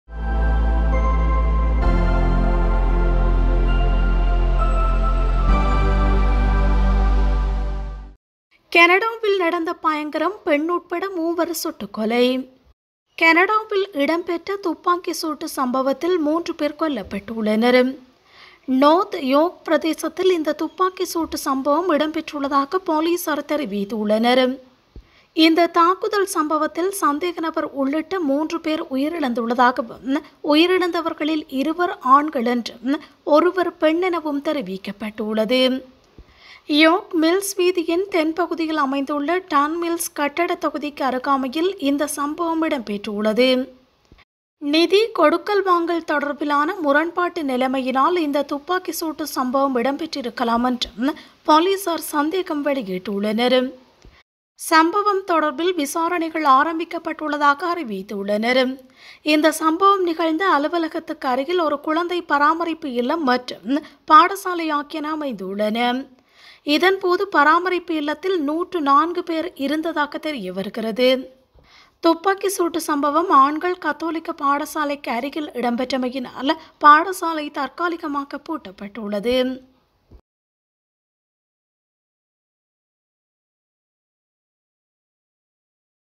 கனடாவில் நடந்த பய*ங்கரம் - பெண் உட்பட மூவரின் நிலை
நோர்த் யோர்க் பிரதேசத்தில் இந்த சம்பவம் இடம்பெற்றுள்ளதாக பொலிஸார் தெரிவித்துள்ளனர்.